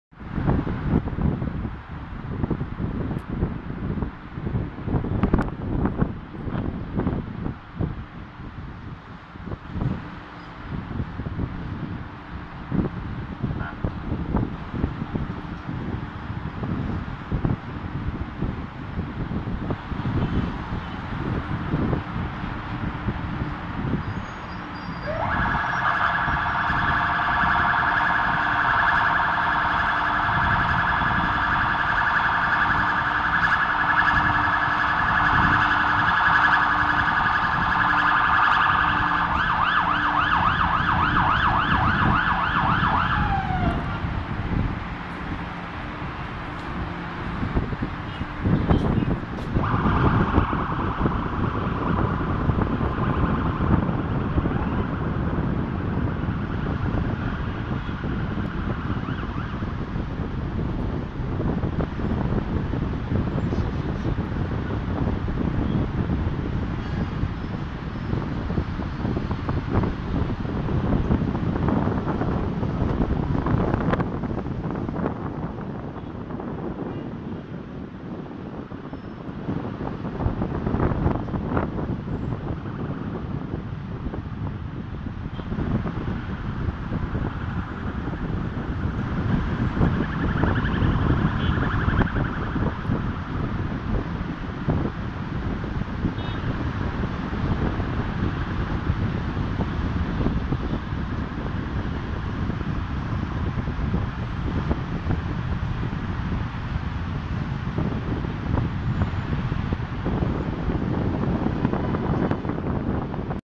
روعة طريق الملك فهد و برج المملكة
تصوير فيديو لطريق الملك فهد و برج المملكة شمال الرياض..

تم التصوير اليوم الاثنين 6/11/2017